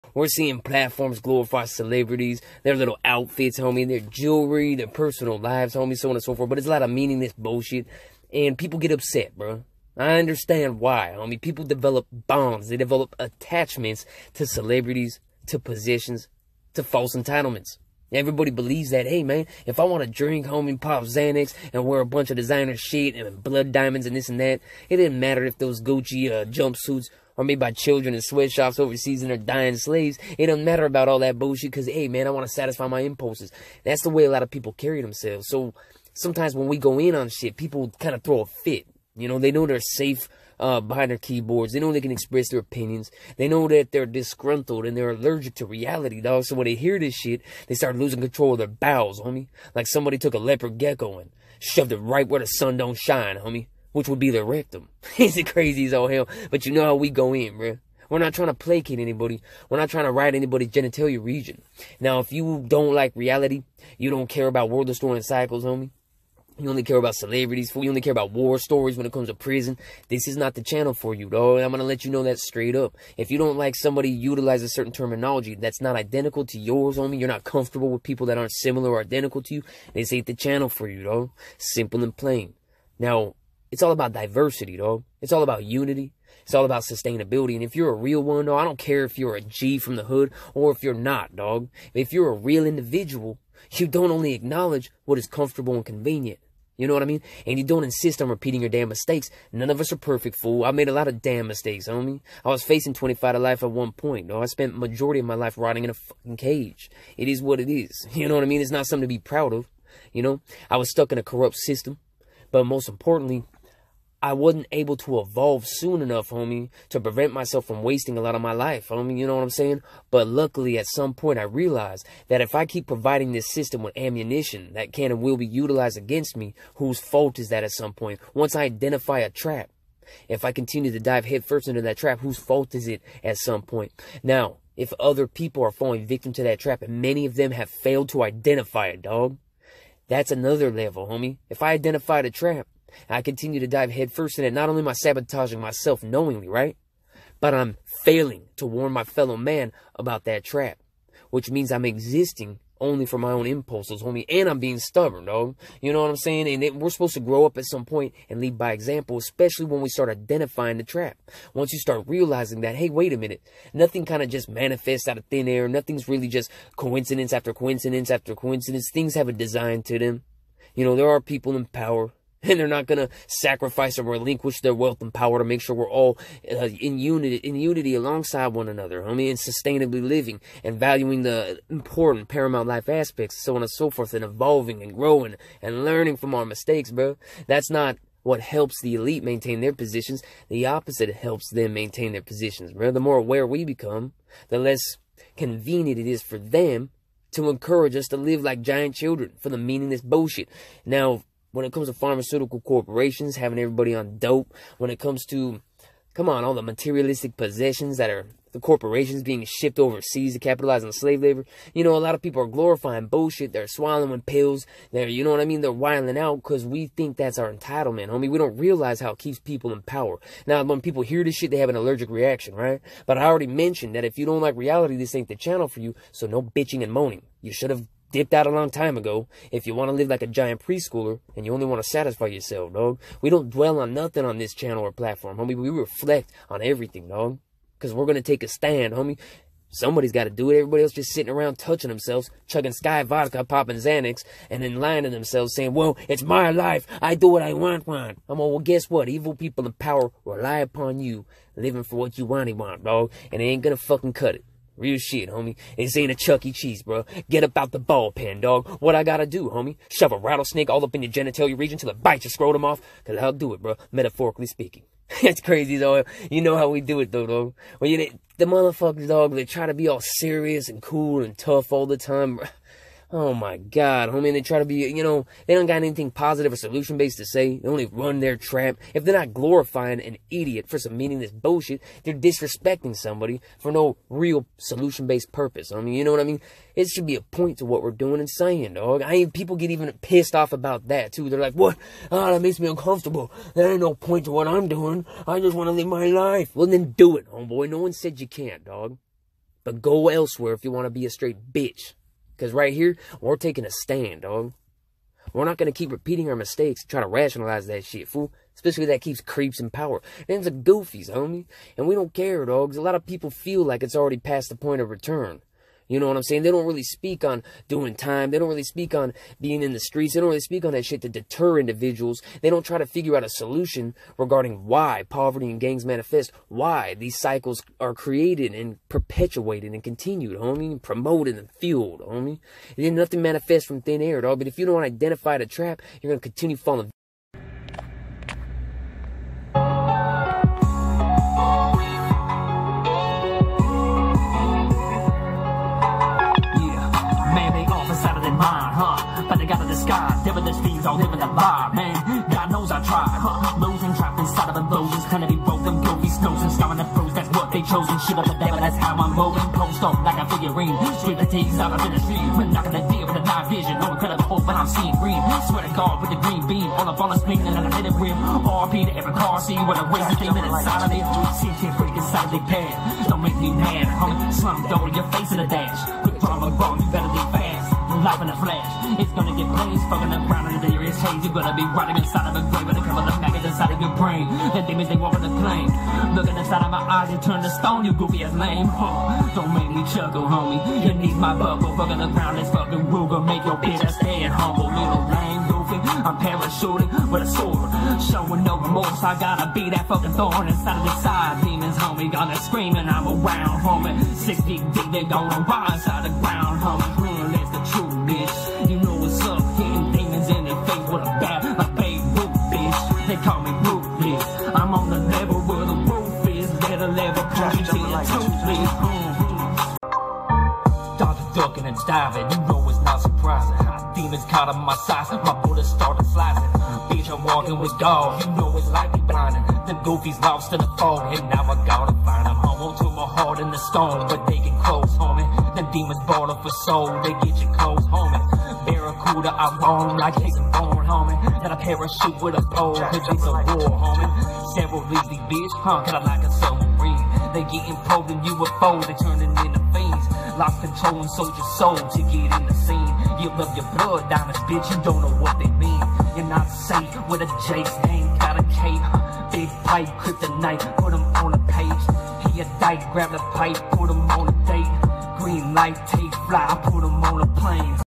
THIS is A Movement NOT A Prison Channel Or Blog About Music & Celebrities!
Had to make this video in response to ignorance. Many people simply don't understand why someone would invest their time & energy in attempting to stop our planet's destruction. It's not "cool" or "popular" whatsoever so most individuals struggle with the concept of learning from their mistakes & continuously evolving. Reality isn't supposed to be avoided & altered, it doesn't have to be our enemy.